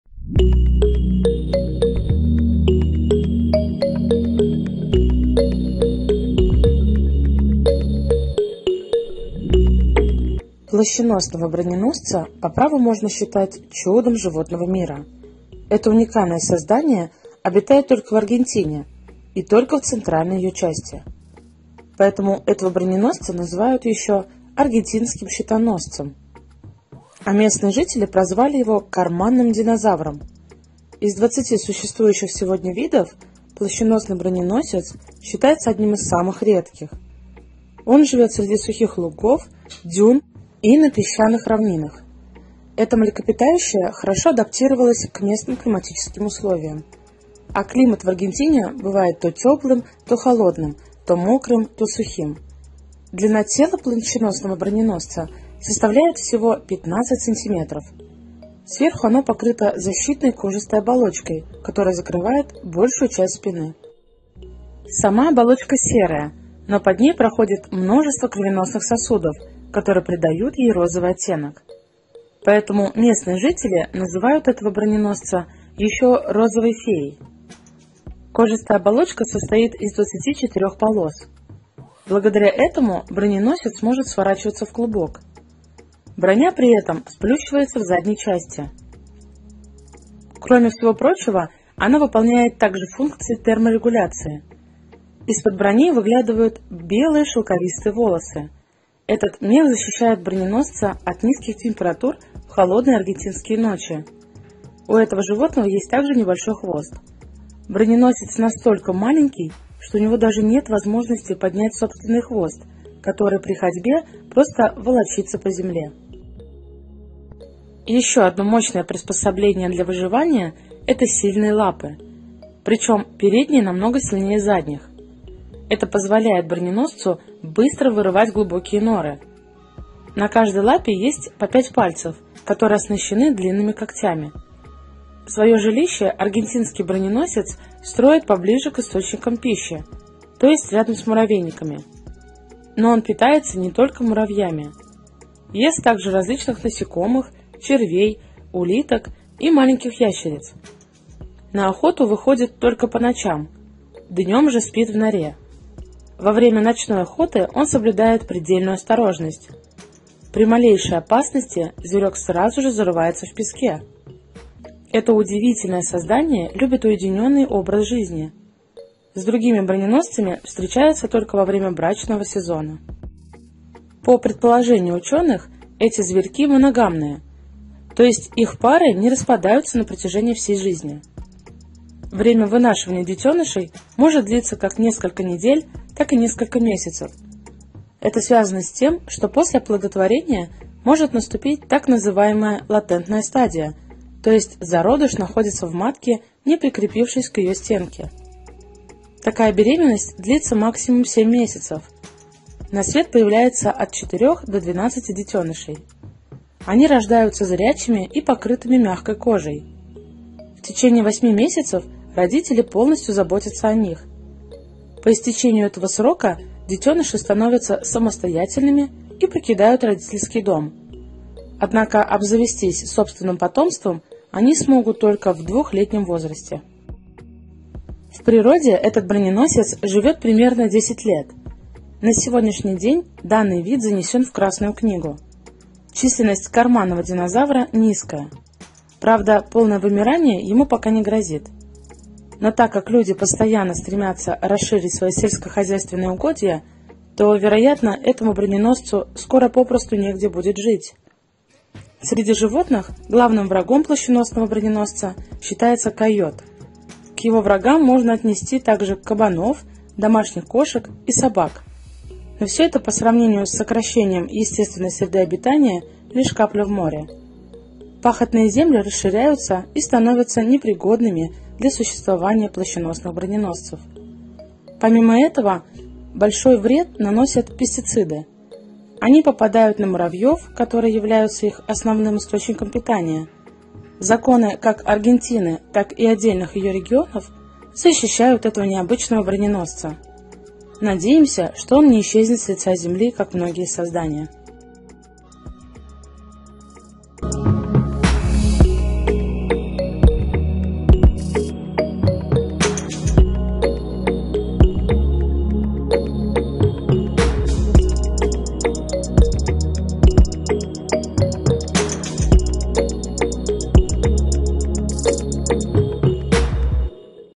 ПЛАЩЕНОСНЫЙ БРОНЕНОСЕЦ - Карманный динозавр аргентинцев
Плащеносного броненосца по праву можно считать чудом животного мира. Это уникальное создание обитает только в Аргентине — и только в центральной её части. Поэтому этого броненосца называют ещё Аргентинским щитоносцем. А местные жители прозвали его «карманным динозавром».